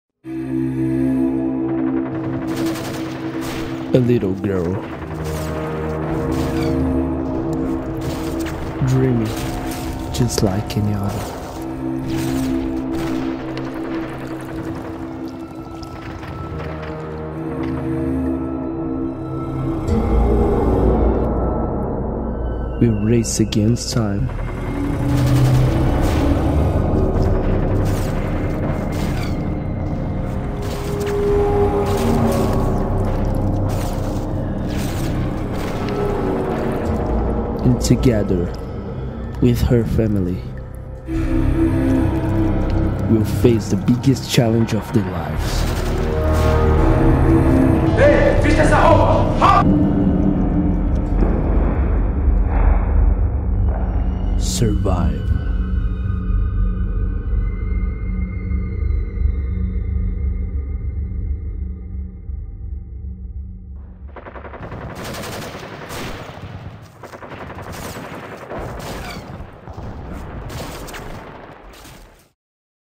Déc 40 e 50 TRAILER UM LUGAR MELHOR
Trabalho desenvolvido pelos alunos da 2° série do Colégio Nossa Senhora das Neves /2015 para as áreas de Humanas e de Linguagens.